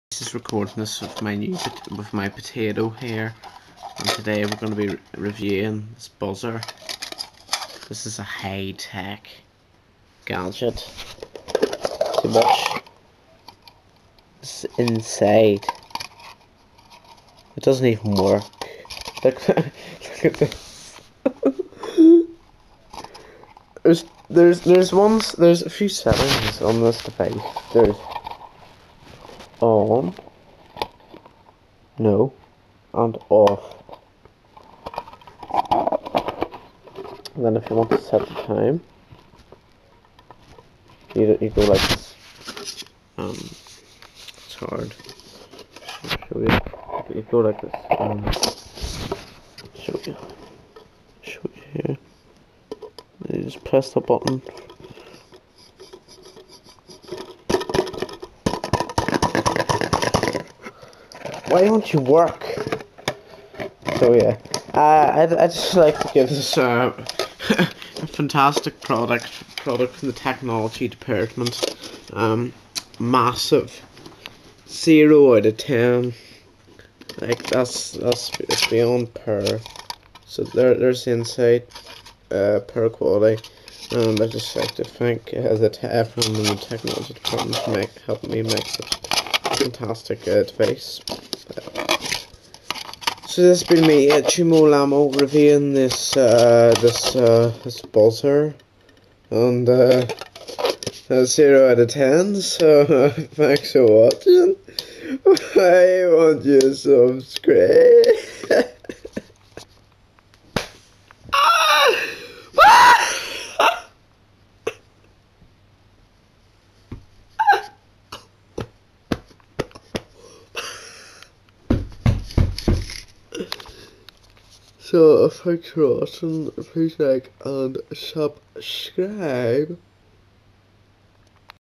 Buzzer Review.
This is me, Tuomo reviewing the Buzzer from Technology.
Credit to:
Technology Department
RegnaCookies